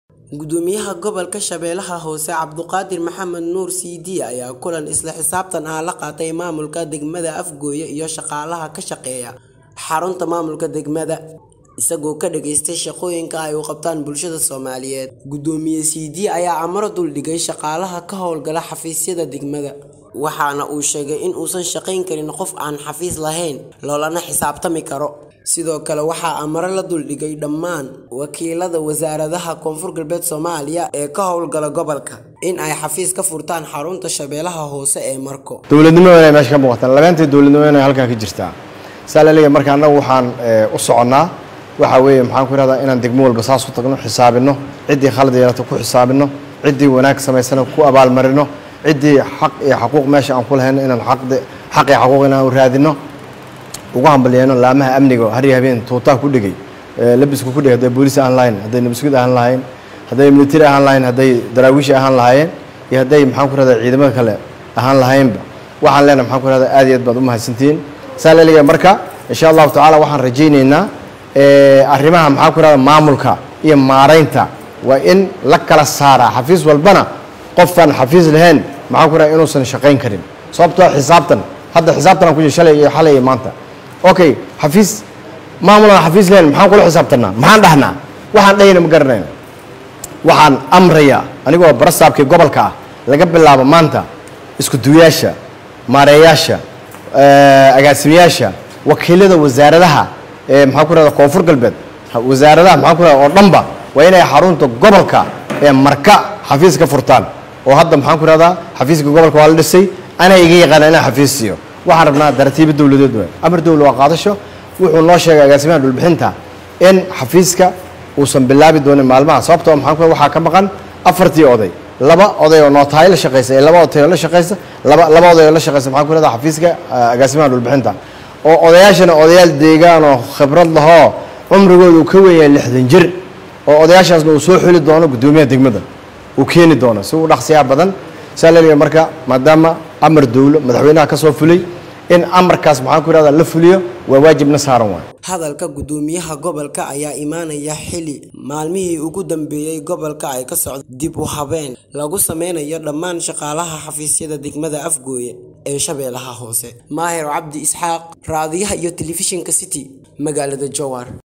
MAAMULKA GOBOLKA SHABELLADA HOOSE OO AMARO DULDHIGAY WAKIILADA WASAARADAHA KG EE KA HOWLGALA........
MAAMULKA GOBOLKA SHABELLADA HOOSE OO AMARO DULDHIGAY WAKIILADA WASAARADAHA KG EE KA HOWLGALA SHABELLADA HOOSE